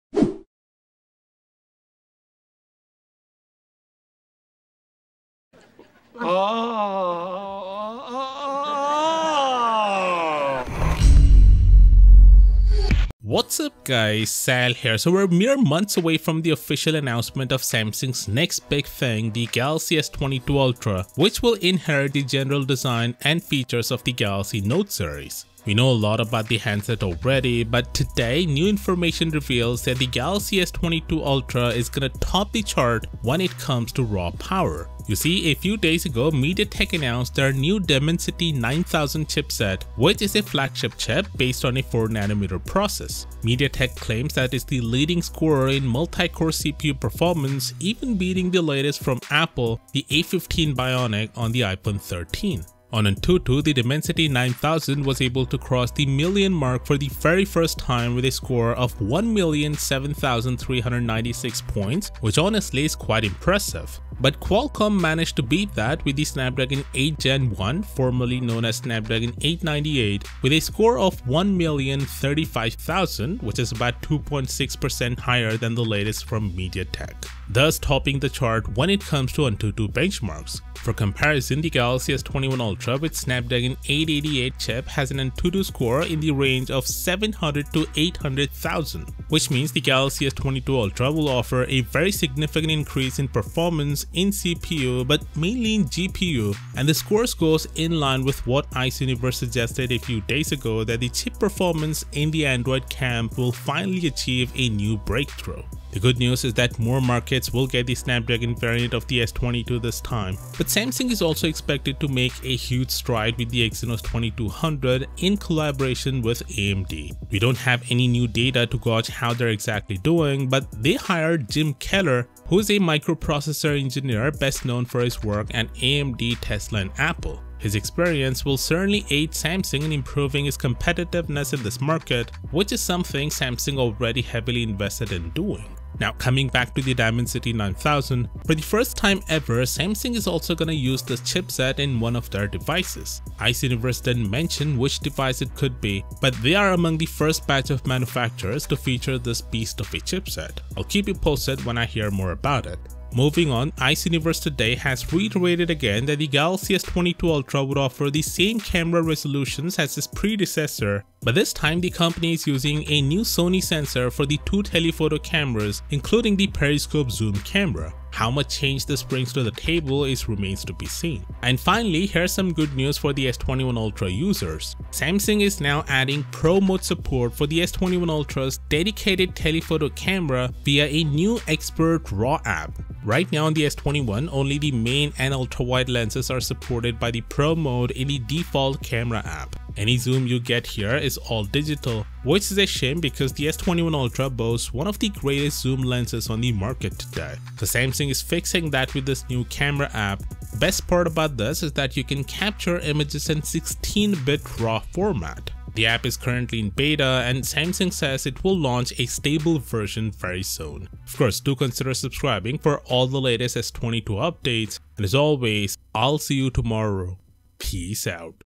Samsung Galaxy S22 Ultra - Exceeding Expectations!
Samsung Galaxy S22 Ultra is going to be the most powerful Android handset when it officially releases in a few months. The Snapdragon 8 Gen 1 powering the Galaxy S22 devices has appeared on Antutu benchmarking site, and it broke all the previous records of the platform. We also have other information about the Galaxy S22 Ultra aka Galaxy Note 22 Ultra including the camera specifications. Samsung will keep a Galaxy S22 Unpacked event on February 8th.